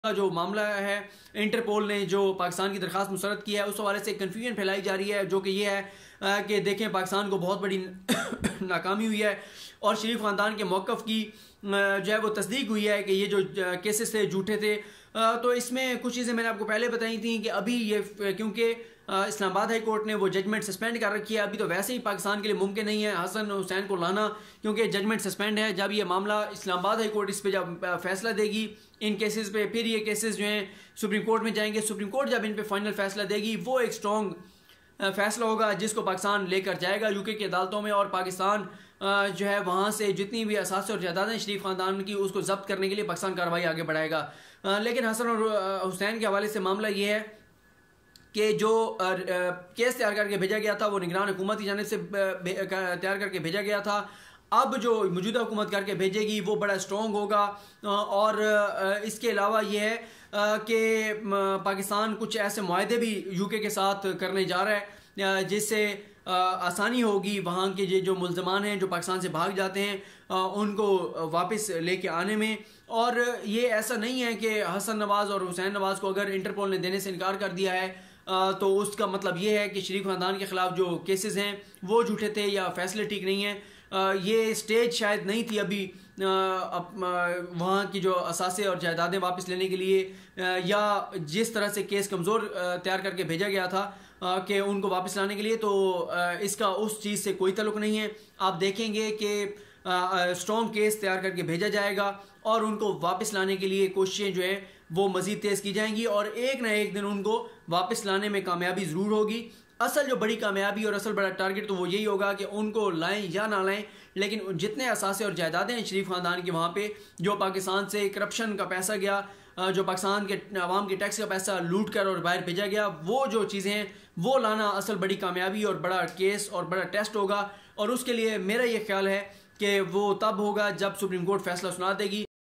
Interpol Refuse to Arrest Hassan & Hussain Nawaz Details By Siddique jan
saddique jan
saddique jaan
siddique jan
sidddique jaan